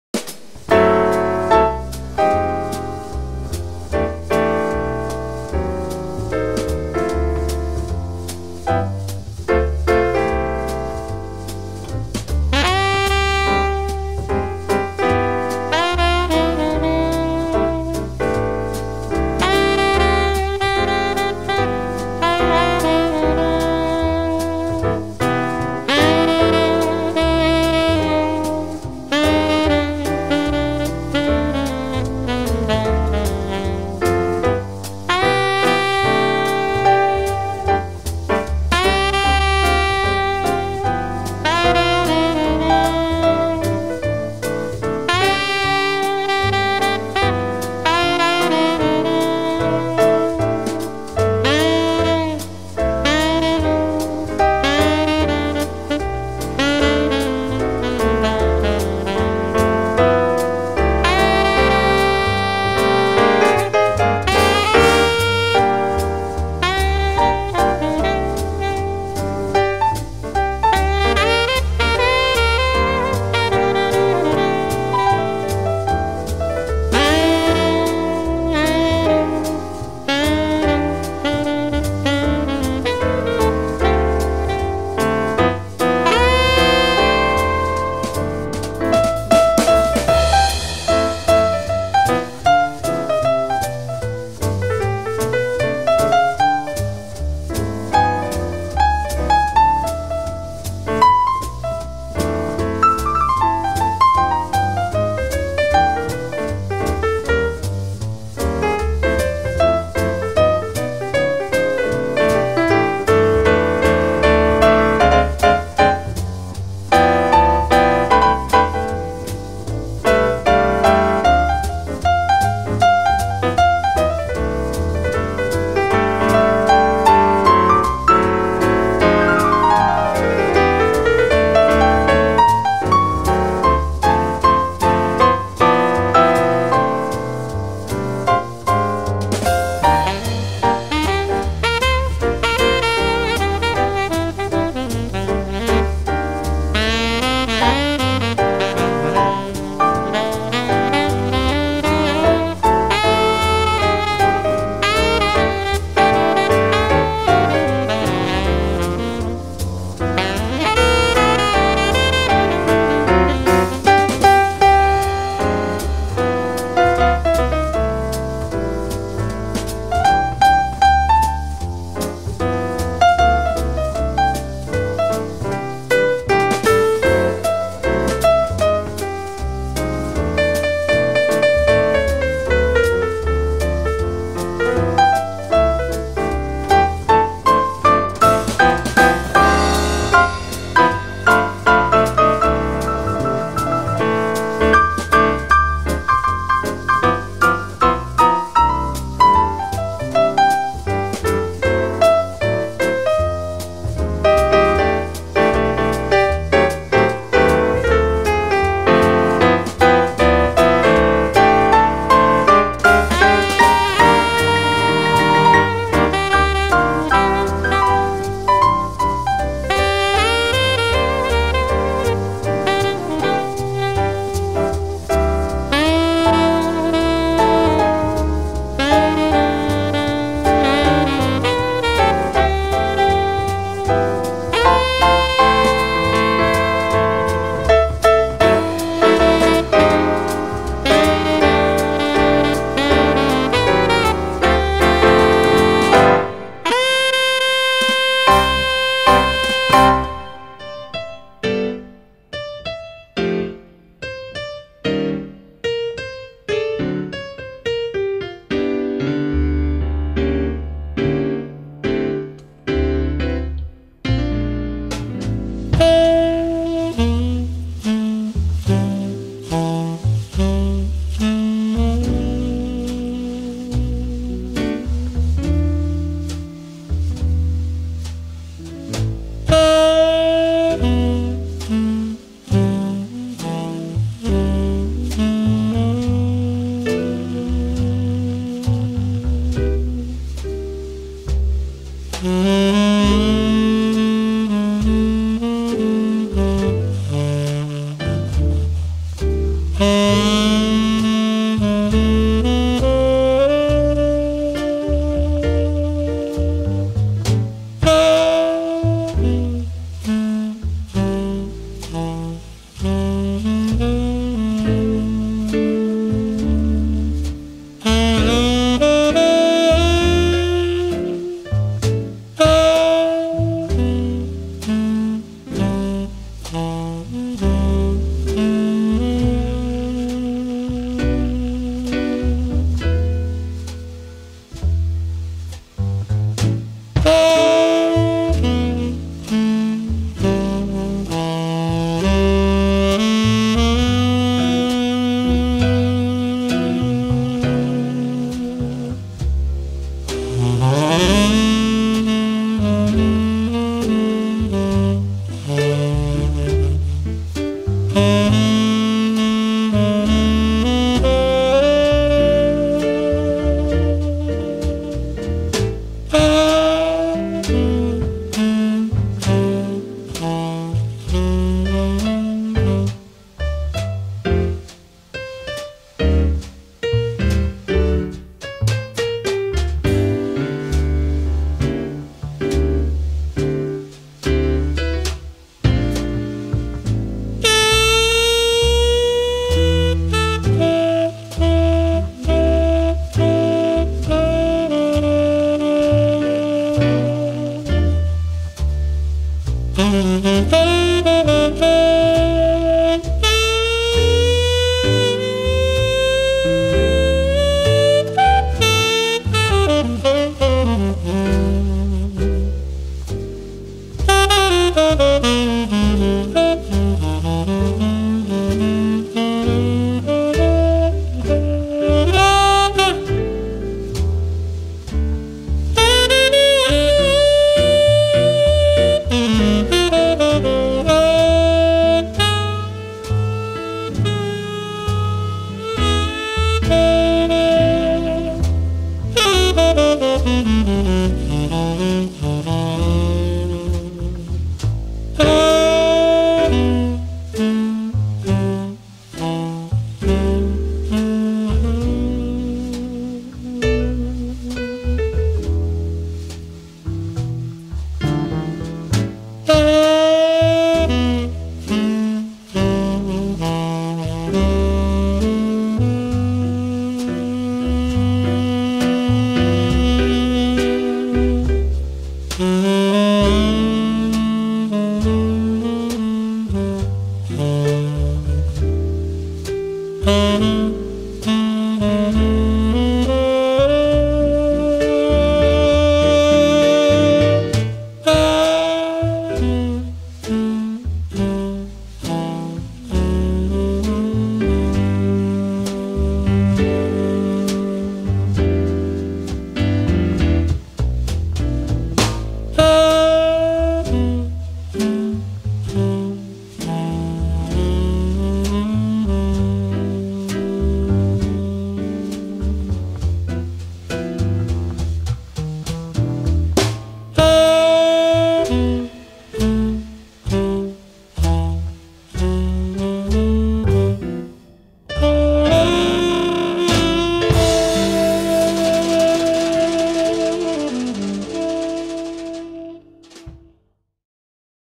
Sant Jordi a casa Roba un poema
ROBA UN POEMA SANT JORDI 2020
En aquest vídeo hem recollit tots els robaunpoema que ens heu fet arribar per celebrar aquest SantJordi2020 des de casa. Esperem que en gaudiu!